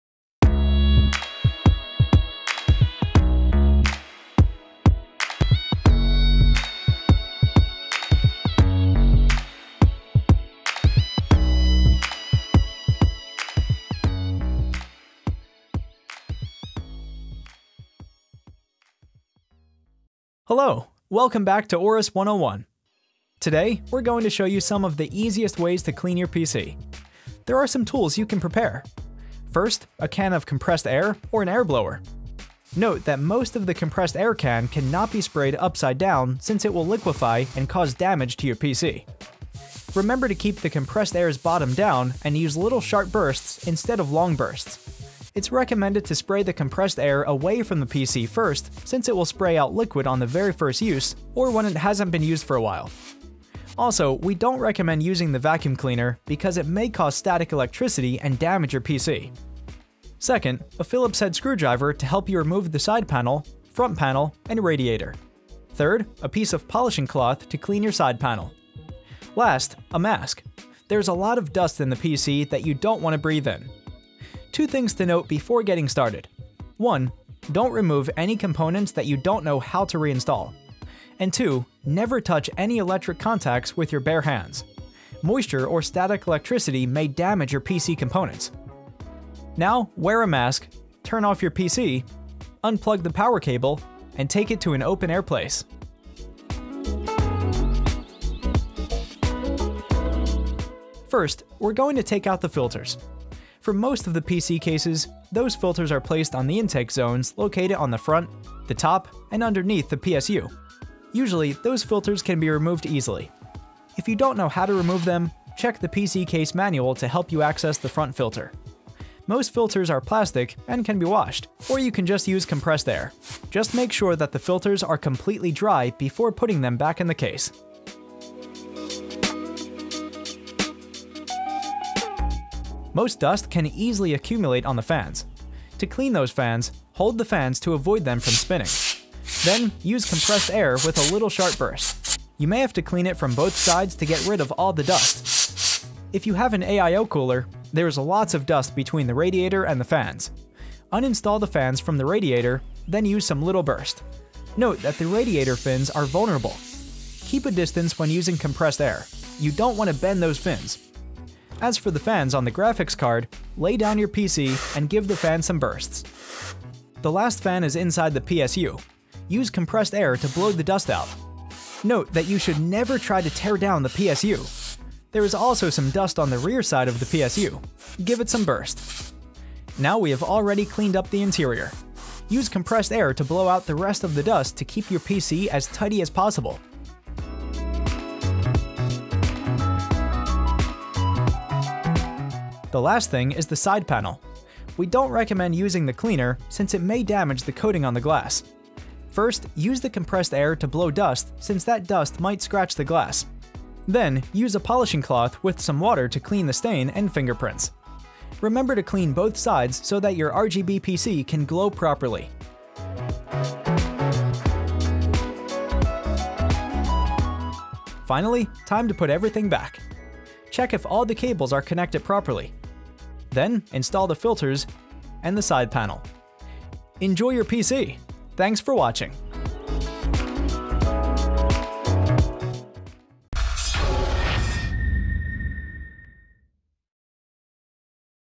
5 bước vệ sinh PC dễ dàng
Cùng AORUS VN tìm hiểu cách đơn giản nhất để làm sạch PC của bạn

1. Những điều lưu ý trước khi tiến hành
2. Vệ sinh lưới lọc bụi
3. Vệ sinh nội thất
4. Làm sạch side pannel
5. Kiểm tra trước khi khởi động

// Theo dõi chúng tôi trên mạng xã hội //
-- Theo dõi chúng tôi trên Facebook: